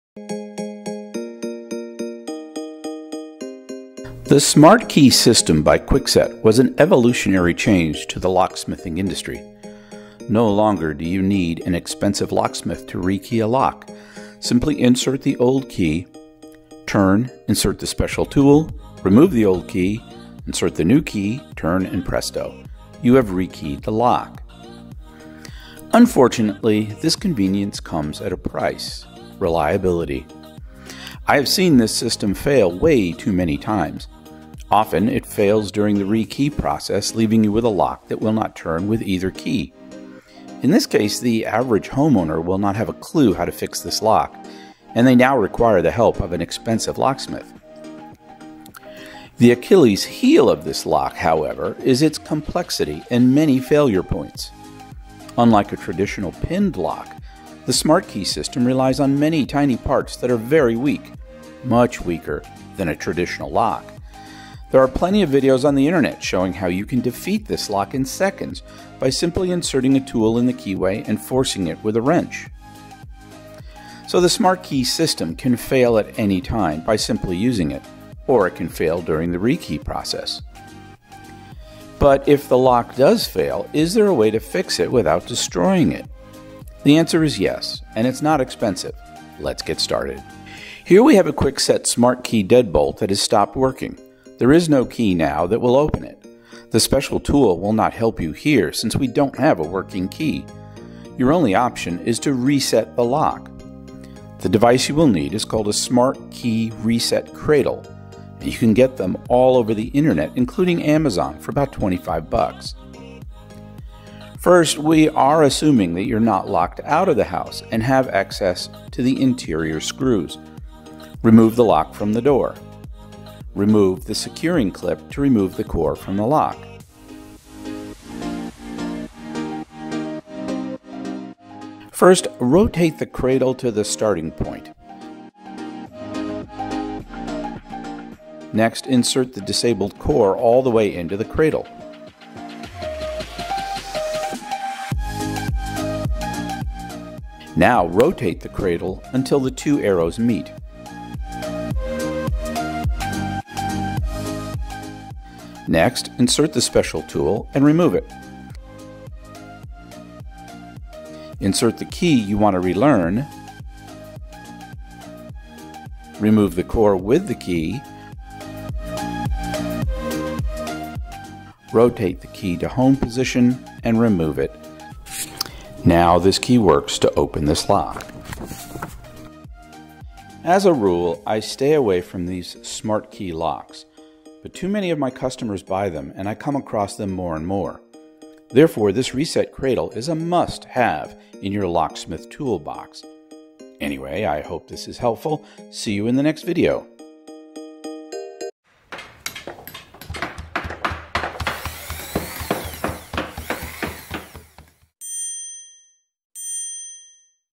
KWIKSET SMARTKEY RESET - HOW TO RESET SMARTKEY
KwikSet SmartKeys are not too smart when they fail. Here's how you "reset" the lock to fix a broken lock.

Link to SmartKey Cradle: